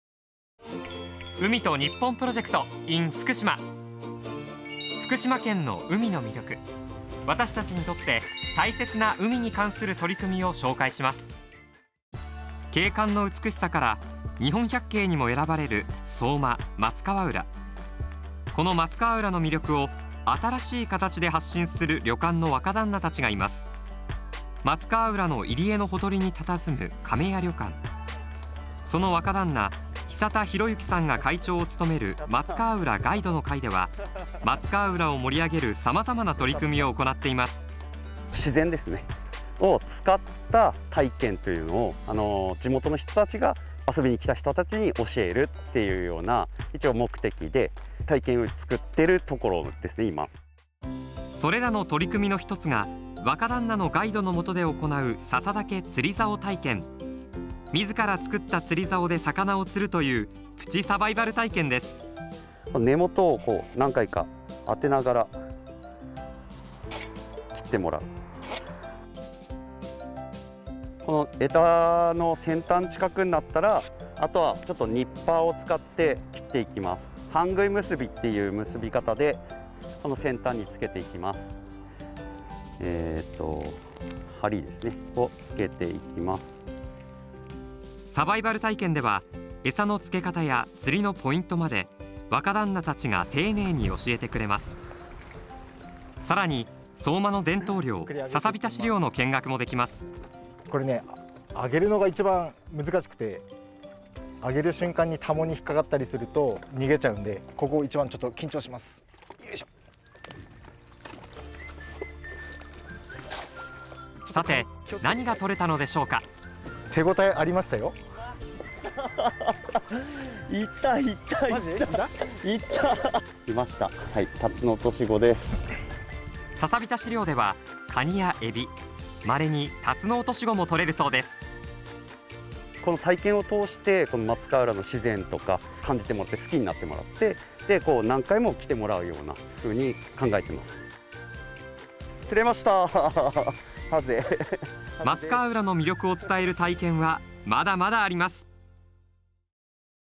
【若旦那のガイド付き】相馬の海を楽しむ宿泊型体験！ 日本財団 海と日本PROJECT in ふくしま 2021 #04
景観の美しさから日本百景にも数えられる相馬松川浦。そんな松川浦の魅力や伝統を繋いでいくために立ち上がったのは地元旅館の若旦那たち。松川浦ガイドの会では会長の久田さんをはじめ会員のみなさんが様々な取り組みを行っています。今回は若旦那のお二人に「笹竹釣竿体験」と「差浸し漁」について教えてもらいました。

・会社名
・部署名
・お名前